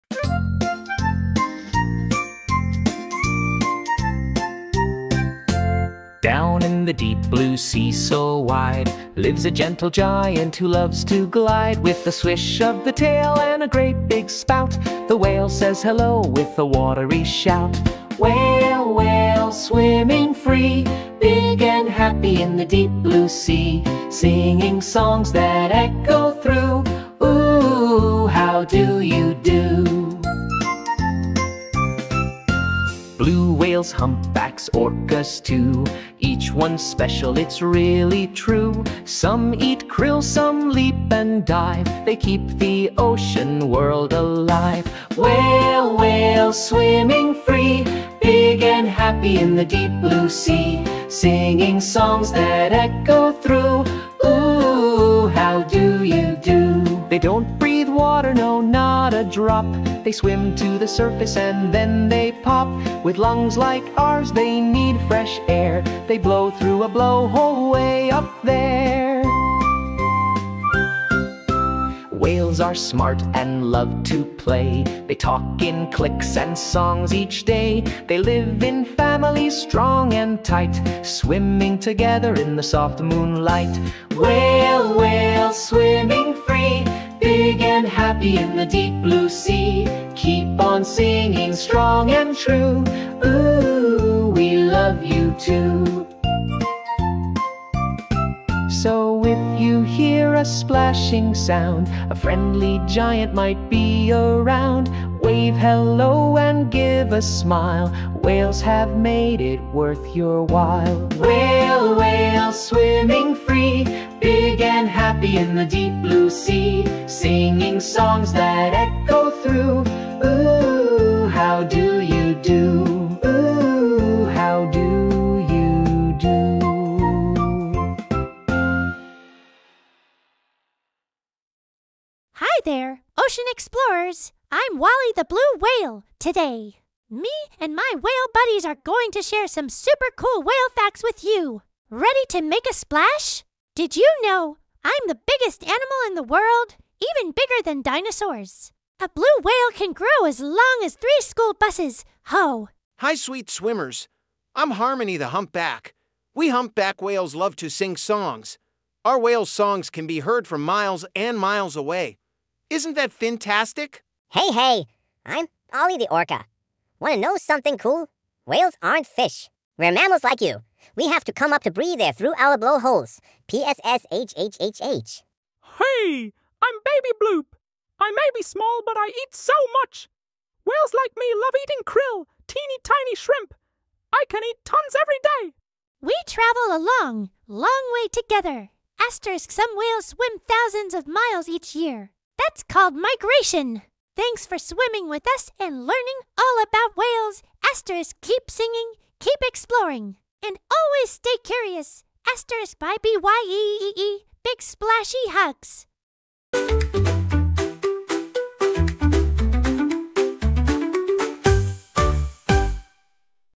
Whale Song and Educational Facts | Animated Video for Children | Cocogigglebeats
Dive into the deep blue sea with Cocogigglebeats in this fun and educational animated video for children!

Meet Wally the Blue Whale, Harmony the Humpback, Ollie the Orca, and Baby Bloop the Beluga as they sing, laugh, and share amazing facts about whales.

Kids will learn:
 • Why whales are the biggest animals on Earth
 • How whales breathe through blowholes
 • What humpback whale songs sound like
 • What krill is and why whales love it
 • How and why whales migrate

Designed for preschoolers and early learners, this video mixes music, fun characters, and real facts in an age-appropriate way to make learning about marine life exciting and memorable.

💡 Your support helps us make more valuable content for kids worldwide.